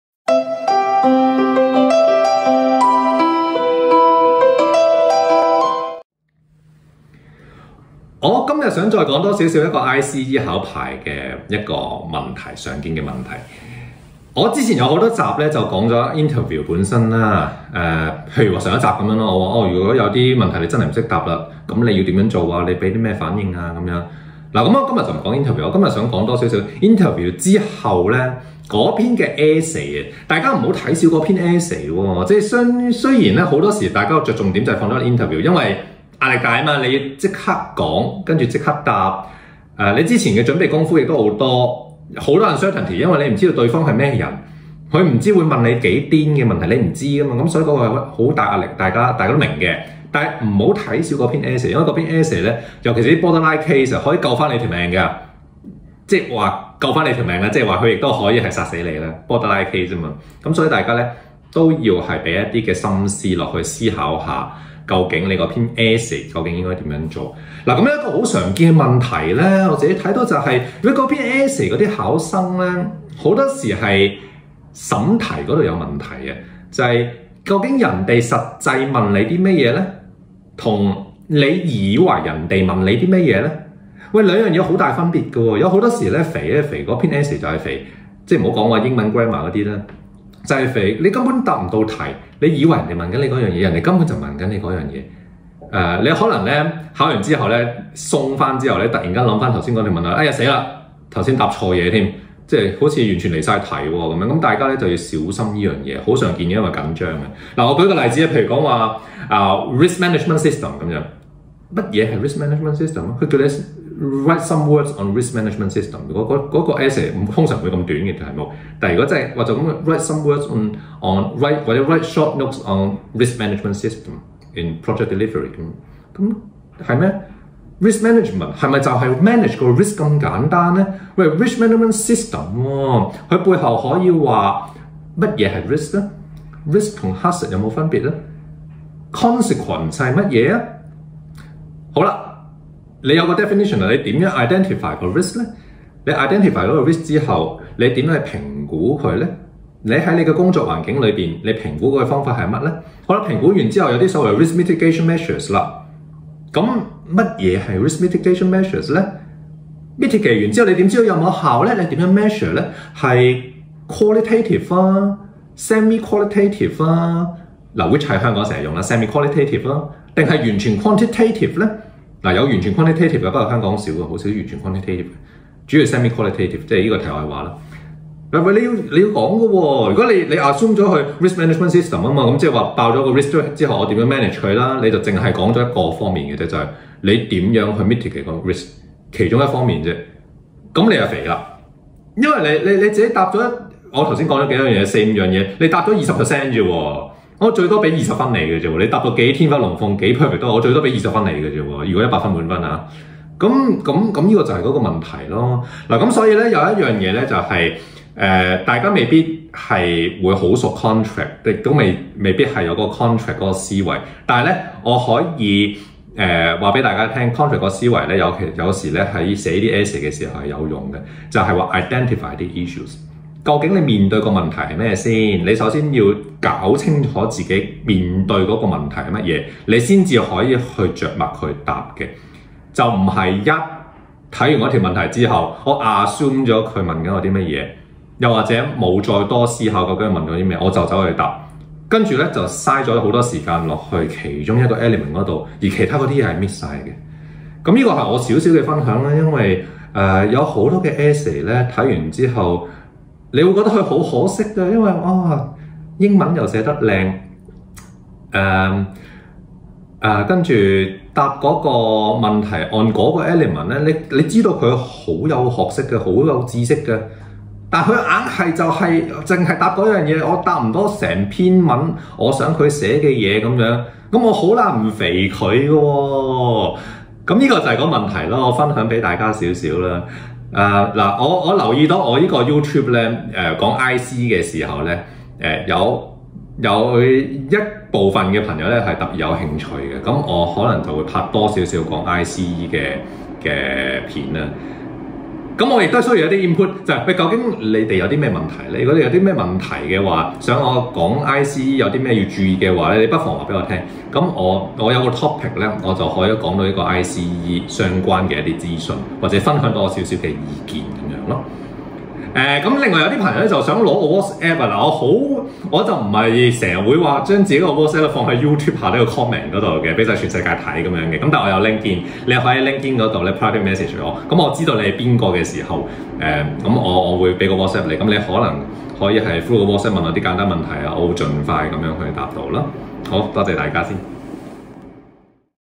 ICE幾分鐘 | CPR: Essay 常見答題陷阱 | Professional Review | 個人意見分享 | Gordon Kwok
In this video, Gordon shares one of the common pitfalls in writing essays in ICE CPR: There is a difference between what you think the question is asking, and what the question is really after. Prepare yourself, build a knowledge base sufficient to understand the issues in front of you will be the key.

Duration: ~5mins
Target Audience: Hong Kong junior engineers taking ICE CPR
Language: Cantonese